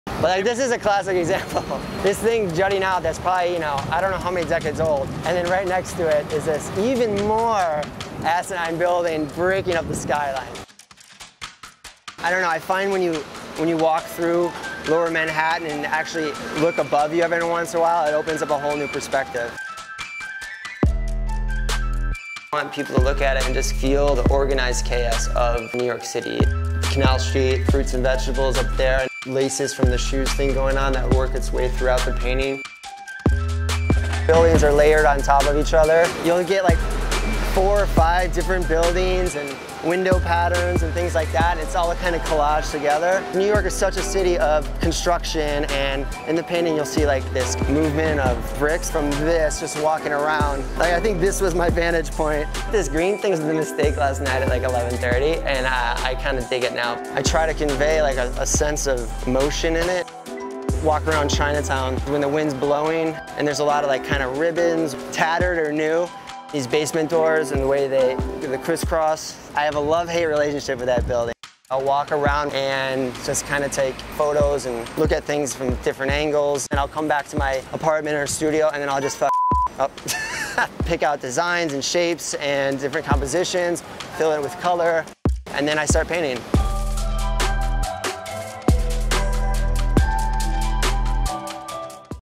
Adam Lucas, Creativity in Process for Adidas originals and Foot Action
Directed by: Giovanni Reda
DP: RB Umali
Agency: Firstborn
Production company: Paige Dorian Productions